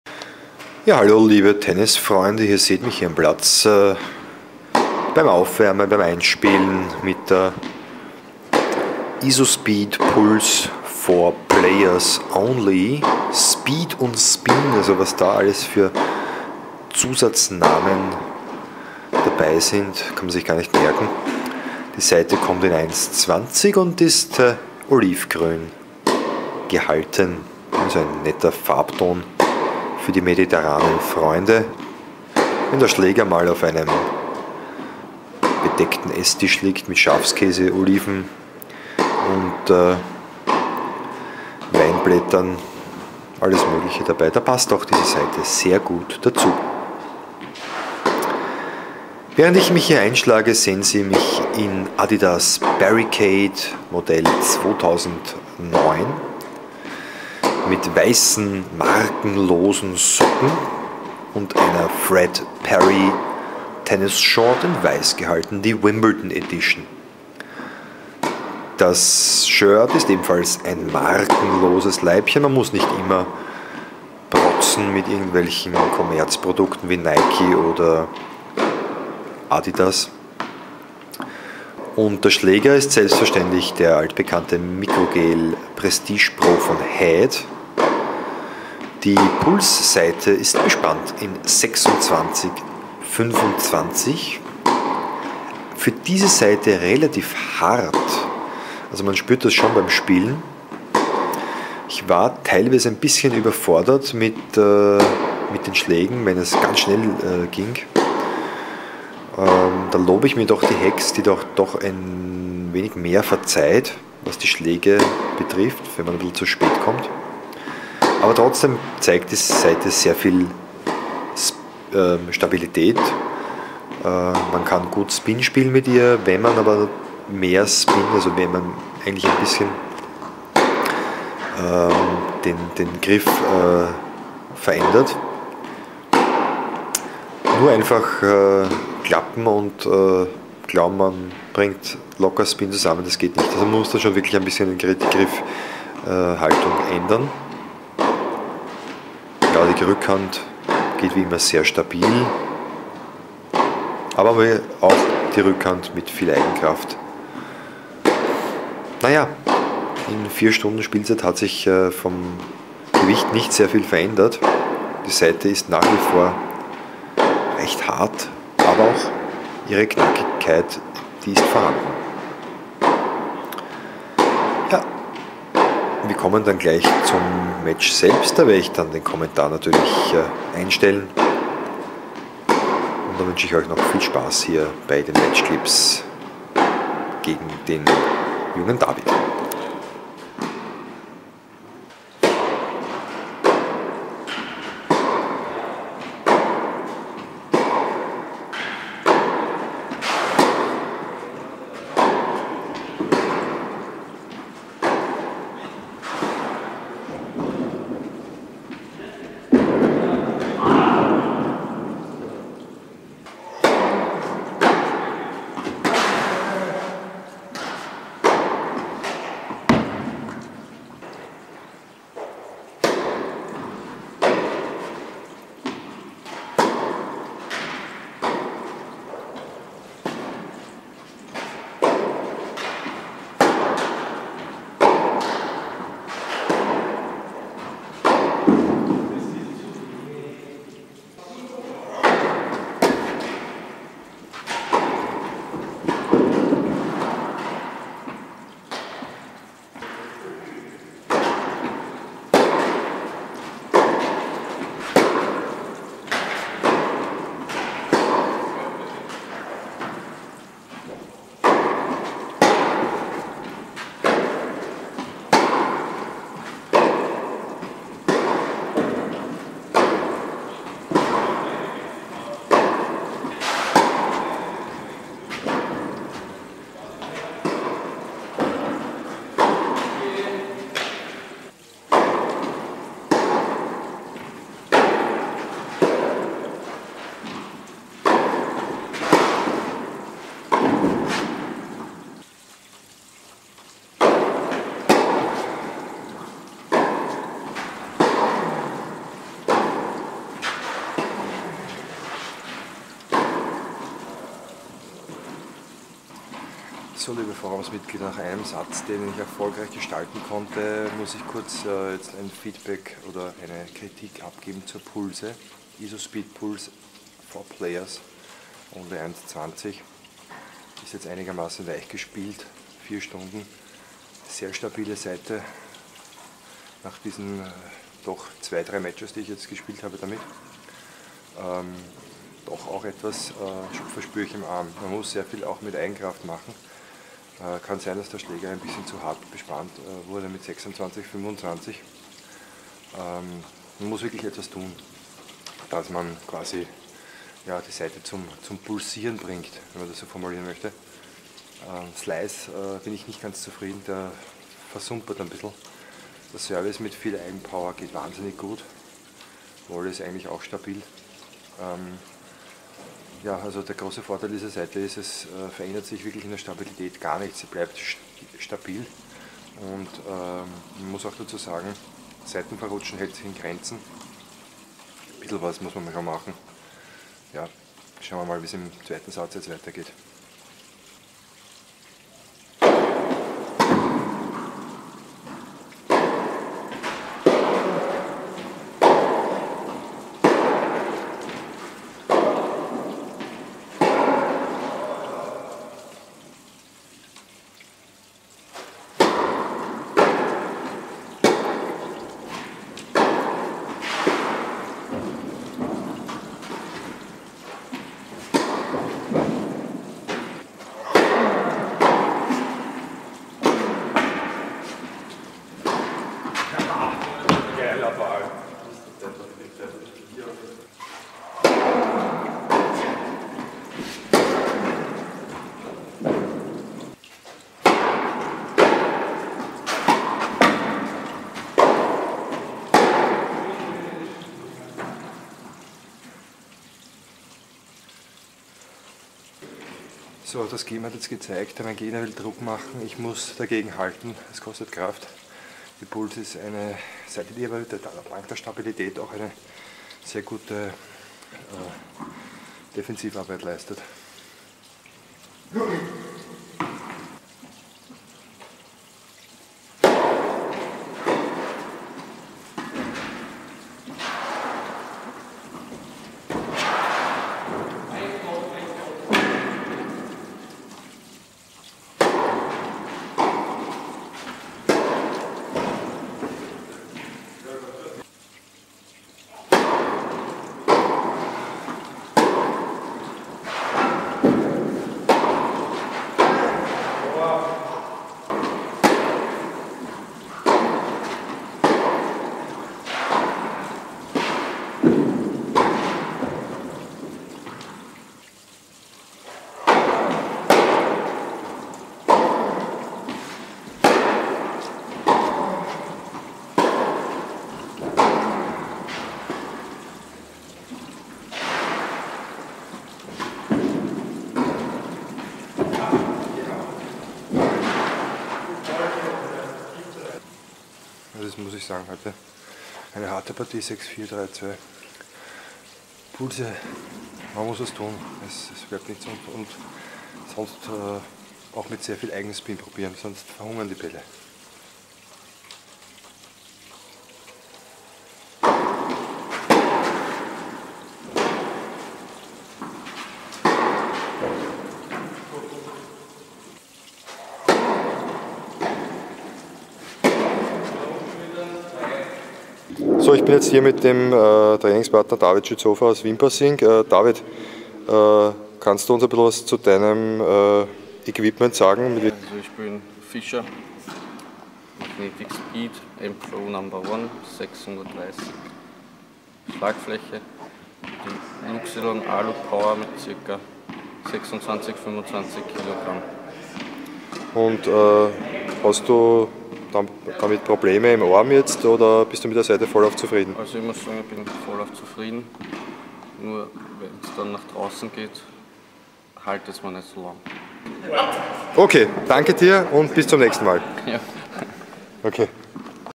DER Saitentester: Isospeed Pulse Speed Spin 4 Players Only 1.20 - VLOG 18
Check out my On Court String Review of the Isospeed Pulse Speed Spin 4 Players Only in 1.20.